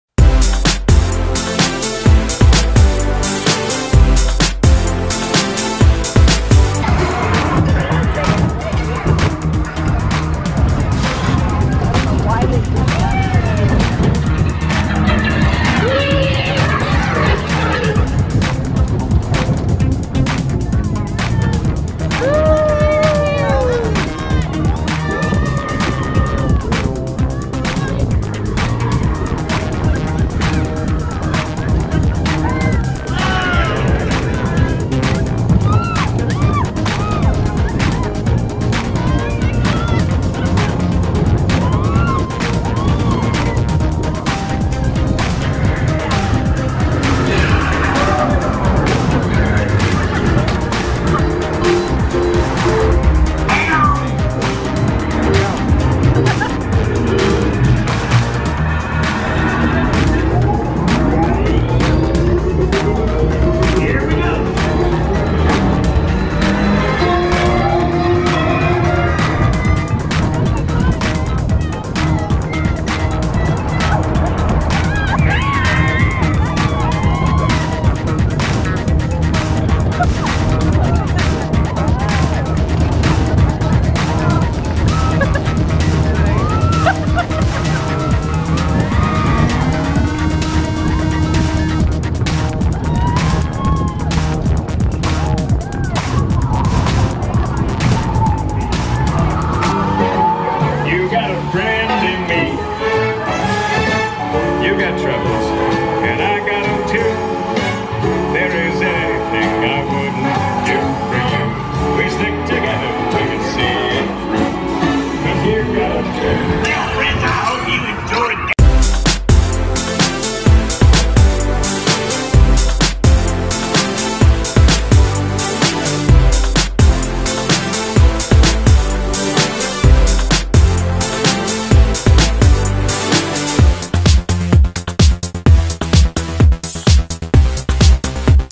Slinky Dog Dash Roller Coaster Front Row PoV in Toy Story Land! Walt Disney World!
Slinky Dog Dash Roller Coaster Front Row PoV in Toy Story Land at Walt Disney World! Full ride through!